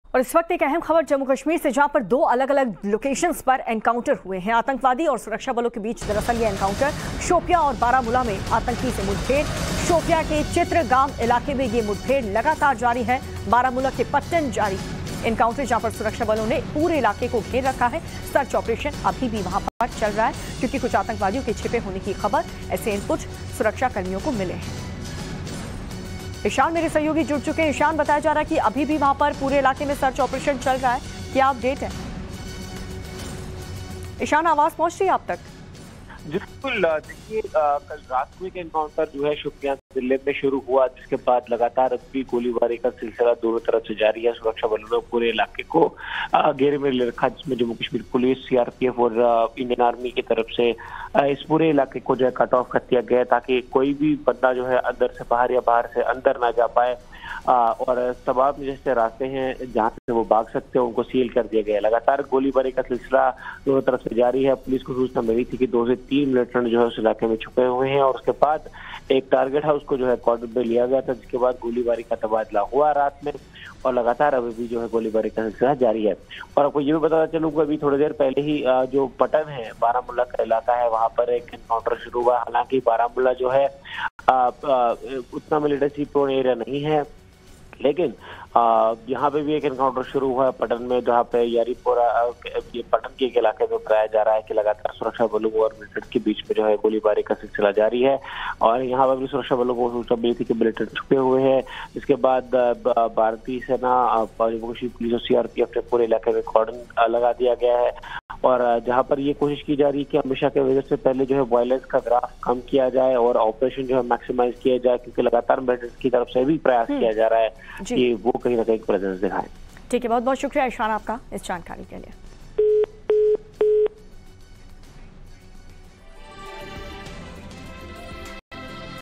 Jammu-Kashmir में दो अलग-अलग जगह पर Encounter | Kashmir Encounter News | Hindi News
Jammu-Kashmir से बड़ी ख़बर आ रही है, जहां पर दो अलग-अलग जगहों पर Encounter हुए है, आंतकियों और सुरक्षाबलों के बीच एनकाउंटर जारी है, Shopian और Chitrgam इलाके में आतंकियों से सुरक्षाबलों की मुठभेड़ जारी है, देखिए पूरी ख़बर...